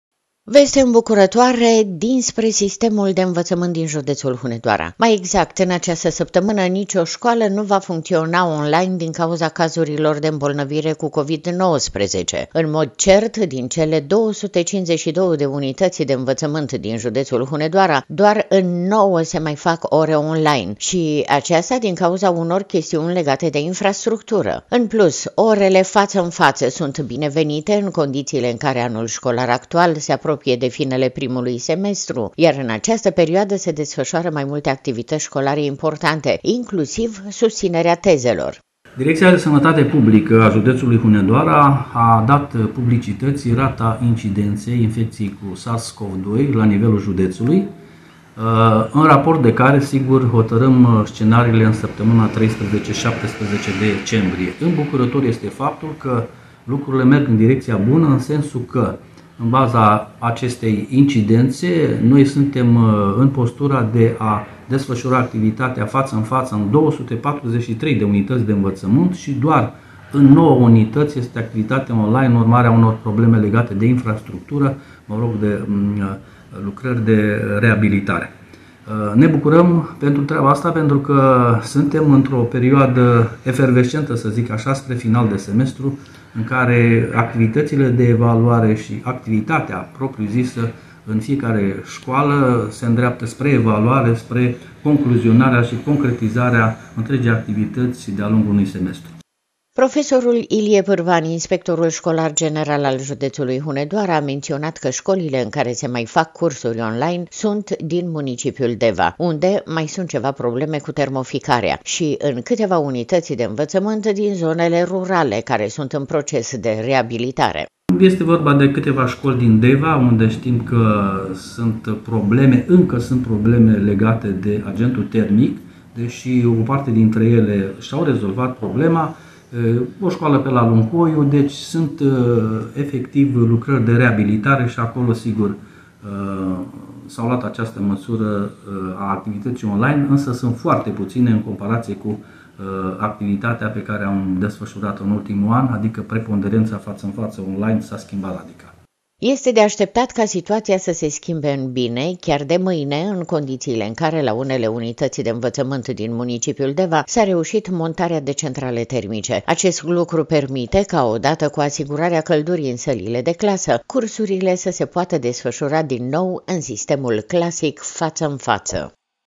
ÎN DOAR NOUĂ ȘCOLI DIN JUDEȚ SE MAI FAC ORE ON-LINE, DAR NU DIN CAUZA PANDEMIEI
Veste îmbucurătoare dinspre sistemul de învățământ din județul Hunedoara. Mai exact, în această săptămână nicio școală nu va funcționa on-line din cauza cazurilor de îmbolnăvire cu Covid 19. În mod concret, din cele 252 de unități de învățământ din județul Hunedoara, doar în 9 se mai fac ore on-line, și aceasta din cauza unor chestiuni legate de infrastructură. În plus, orele față în față sunt binevenite, în condițiile în care anul școlar actual se apropie de finele primului semestru, iar în această perioadă se desfășoară mai multe activități școlare importante, inclusiv susținerea tezelor.